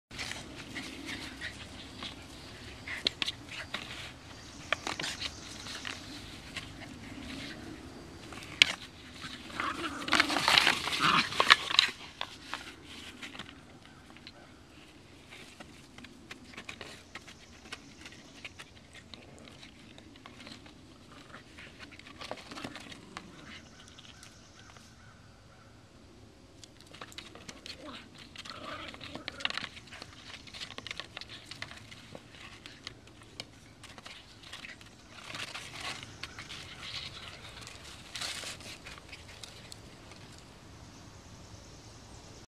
Border Terrier Puppy Playing with Construction Tube
Our little 9 month old BorderTerrier has fun with a construction tube.  We are in the process of doing some back yard projects and Kally found a great new toy with the left over construction tube...Lots of Border Fun!!!